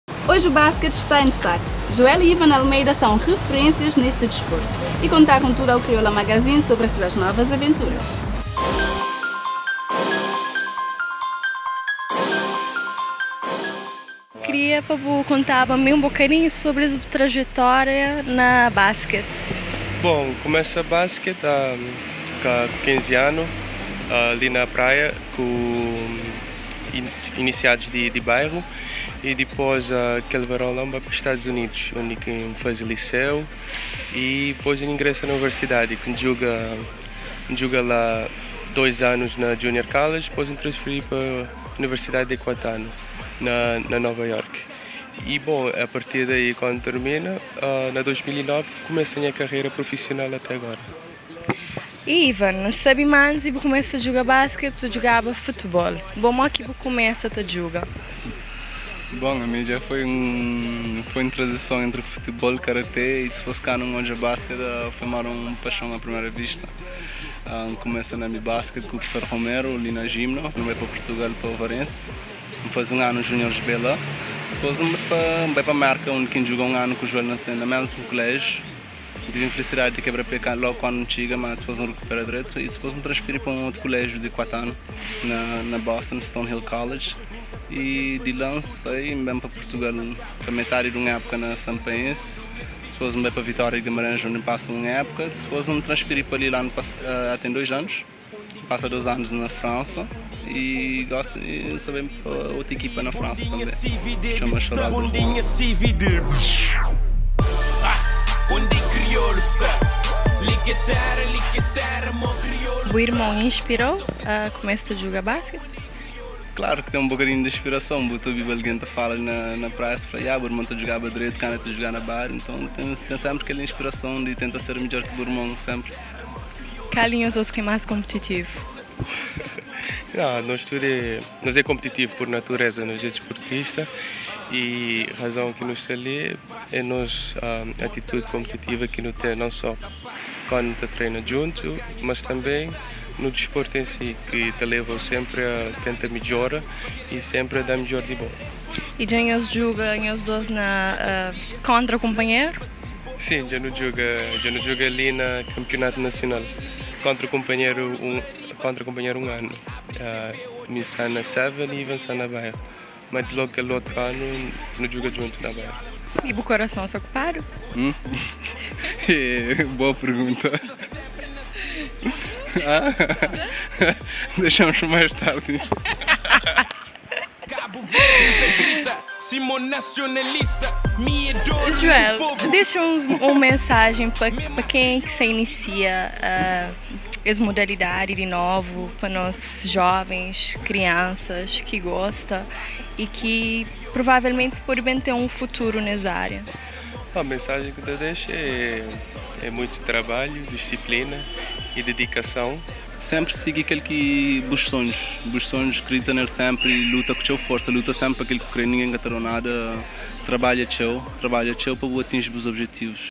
KM10 - Irmãos Almeida
Joel e Ivan Almeida são referências no mundo do Basket e contaram tudo ao Kriola Magazine sobre o seu percurso!